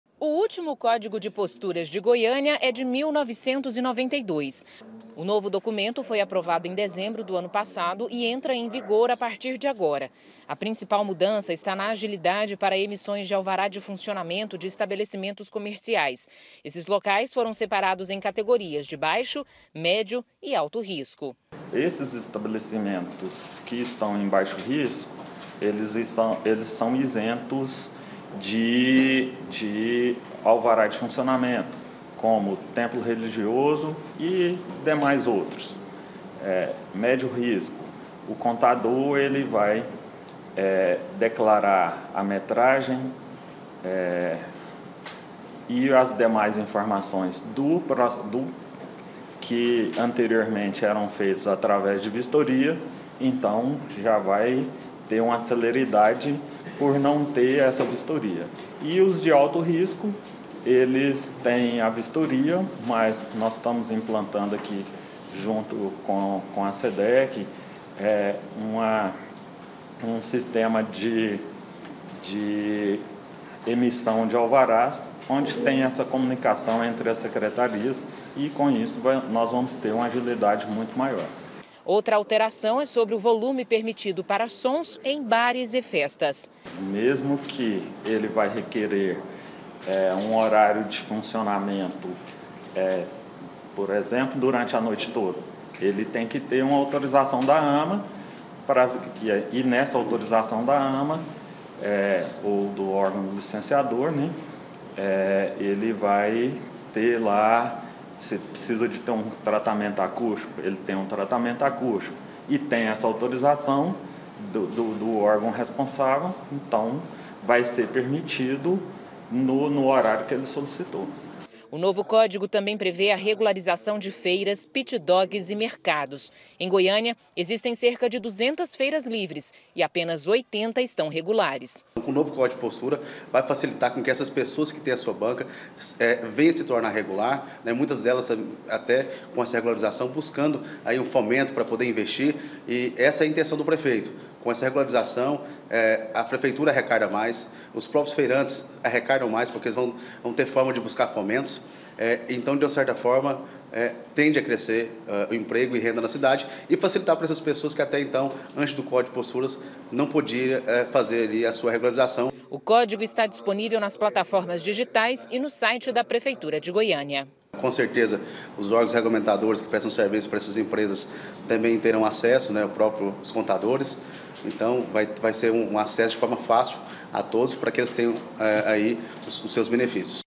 JÁ ESTÁ VALENDO! NOVO CÓDIGO DE POSTURAS DE GOIÂNIA TRAZ MUDANÇAS
JÁ ESTÁ VALENDO O NOVO CÓDIGO DE POSTURAS DE GOIÂNIA. A LEI FOI APROVADA PELA CÂMARA DE VEREADORES, EM DEZEMBRO DO ANO PASSADO, DEPOIS DE MAIS DE TRINTA ANOS SEM ATUALIZAÇÃO.